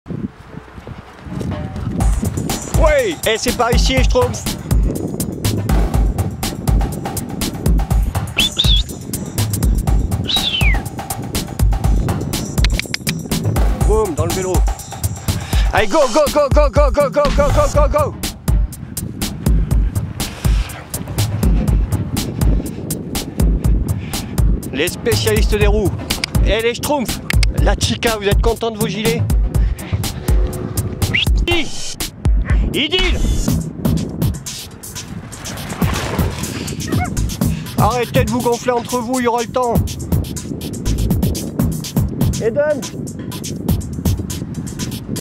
DU JOYAU DES BOIS J -1 7 11 2018 (6.11.2018)
Essaie de nouveau gilet de visibilité...
Visiblement les gilets casaques ne les gènent pas pour courir mais ça les incite peut-être un peu à déconner entre eux. Les débuts de balades sont souvent comme ça surexcité les chiens attendent qu'on aillent se régaler à vélo...